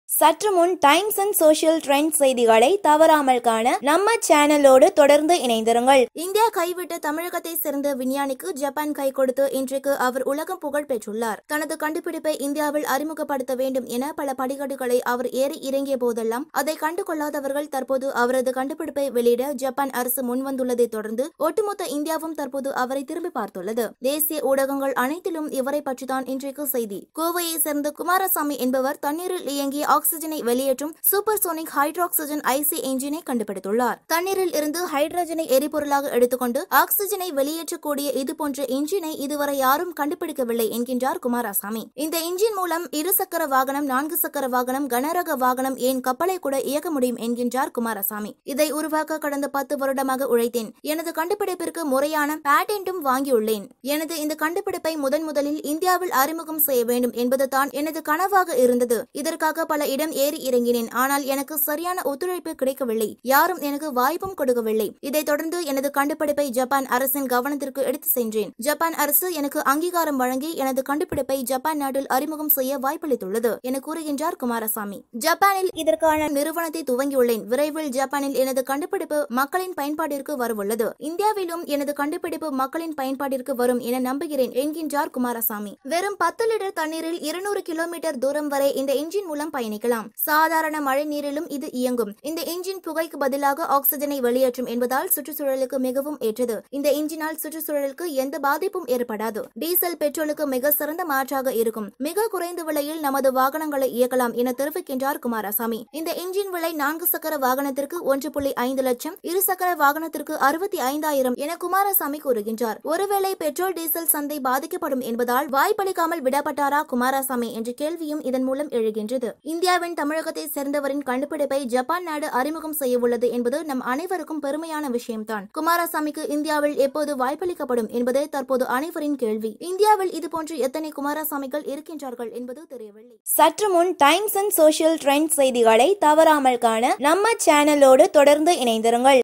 உலகில் இதுவரை யாரும் கண்டுபிடிக்காத பொதுமக்களின் செலவை பெரிதும் குறைக்கும் கண்டுபிடிப்பு
mechanical engineer kumarasamy from Coimbatore tamil nadu invented ”Super Sonic Hydrogen IC Engine" uses hydrogen as fuel, exhausts oxygen.